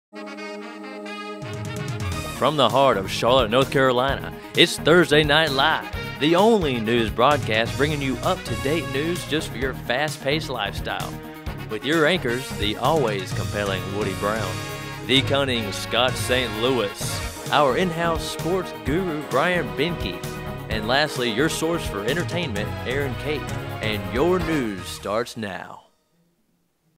Promo For My Newscast - Thursday Night Live
30 Second Promo for the Thursday Night Live Newscast that I co-anchored as an assignment while I was a student at the Connecticut School of Broadcasting.